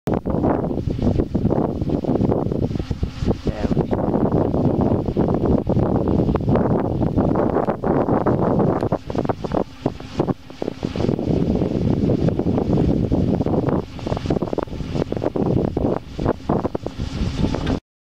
MCS Kestrel 3.5kW e400nb Wind Turbine spinning away in Devon, UK
Helping a UK farmer reach self sustainability. A deep cycle battery stores energy for low wind days. Minimal maintenance, simple and reliable. MCS..
South Devon, UK